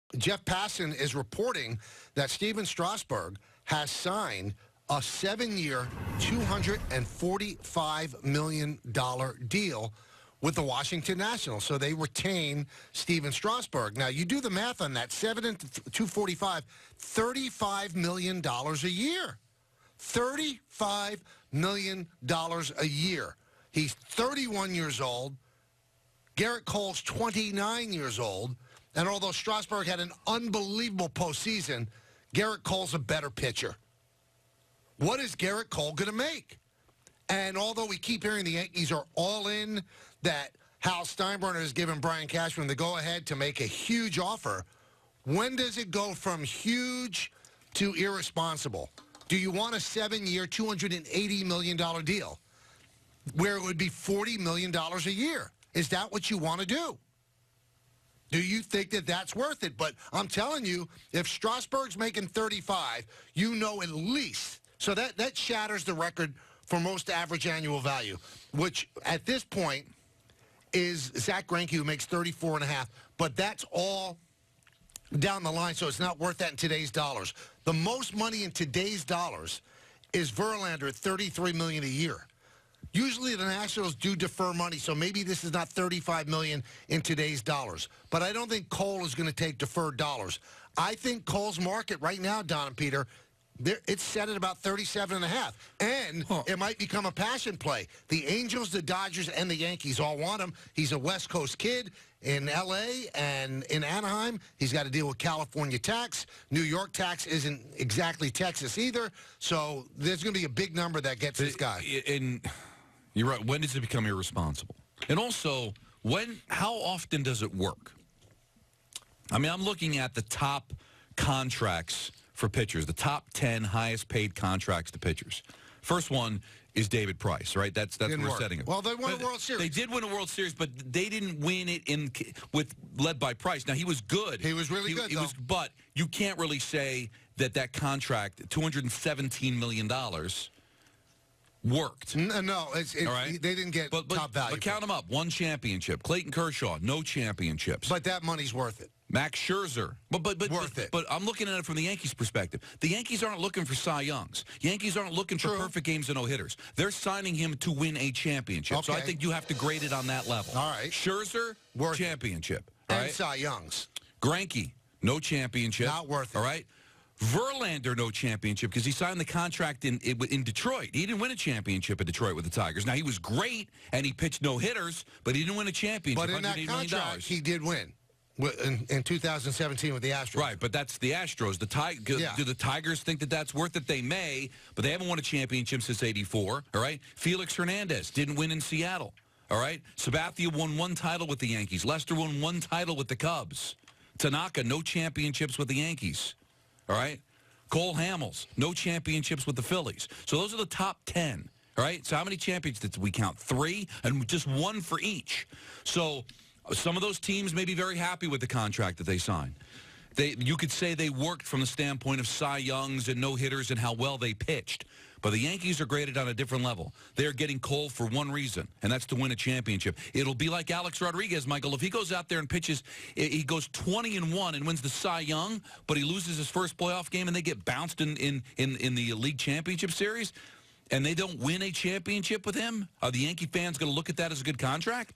Report: Stephen Strasburg re-signs with Nats for 7 years, $245 million
Stephen Strasburg is now the highest-paid free agent in MLB history after reportedly signing a new deal with the Washington Nationals. What could that mean for the pursuit of Gerrit Cole in the grand scheme of things?